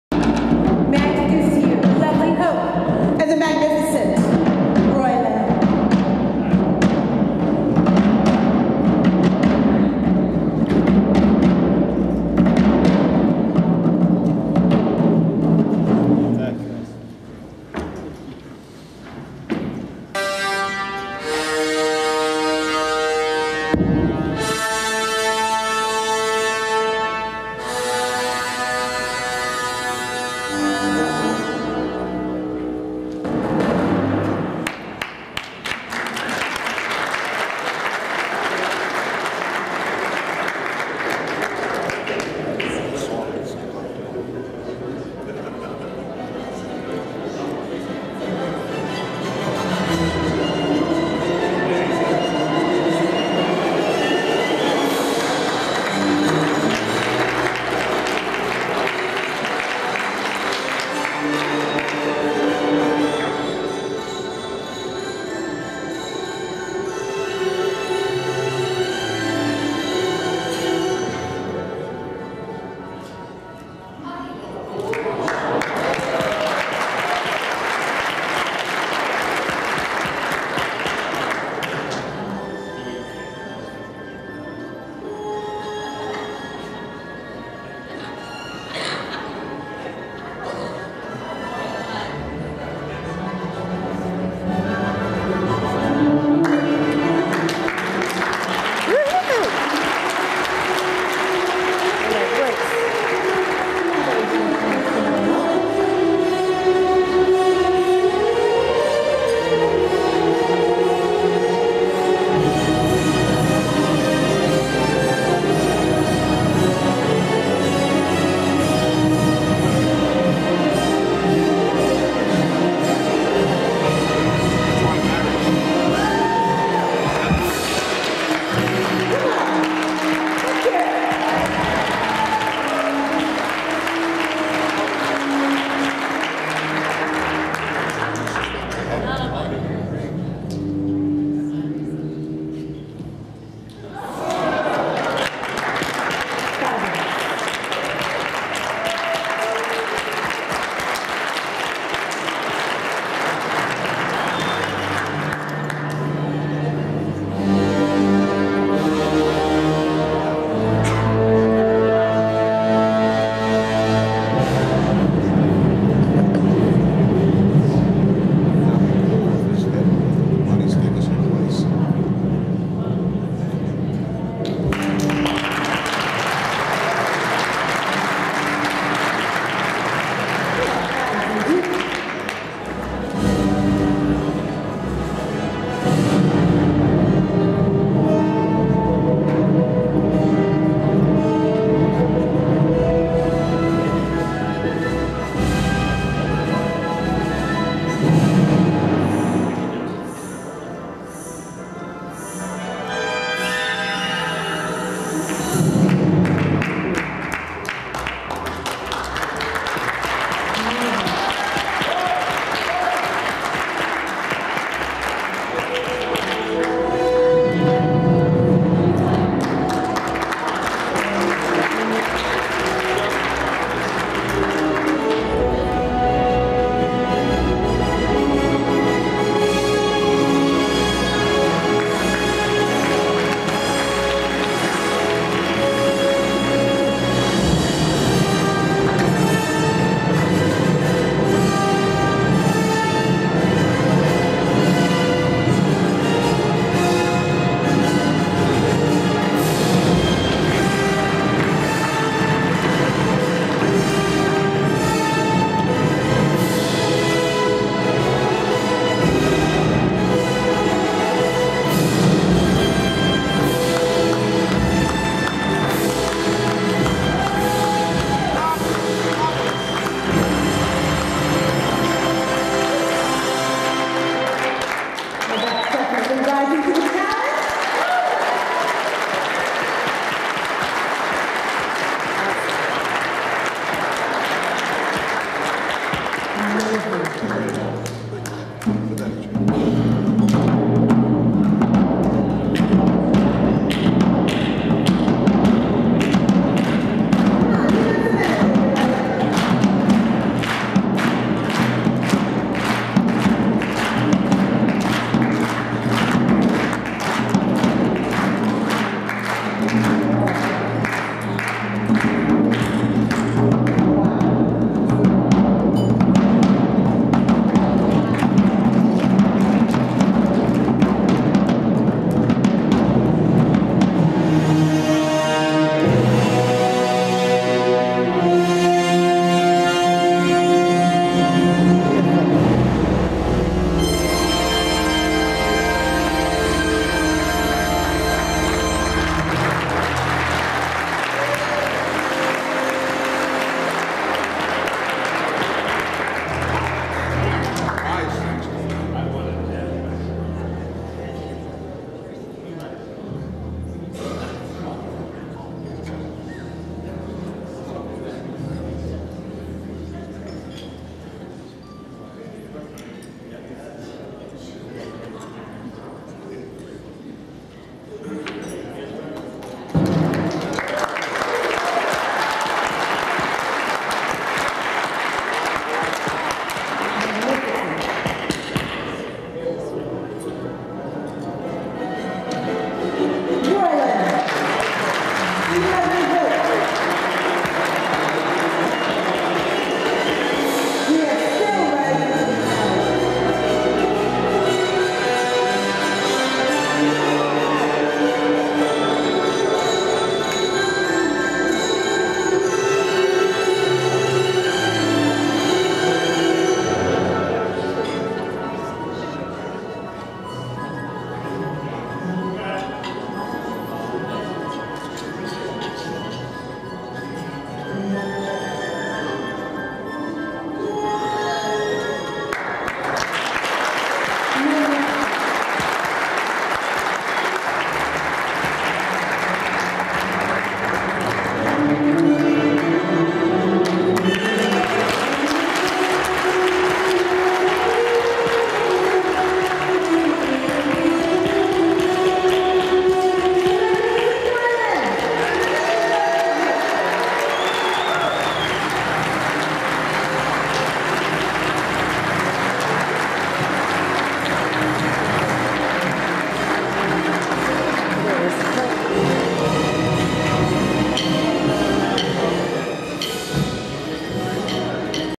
Extreme Handbalancing Roilan Rise up to the Challenge
Номер выдвинут на соискание премии «Мастер» в номинациях «Номер года», «Артист года», «Аттракцион года», «Цирковое шоу года». Подробности на сайте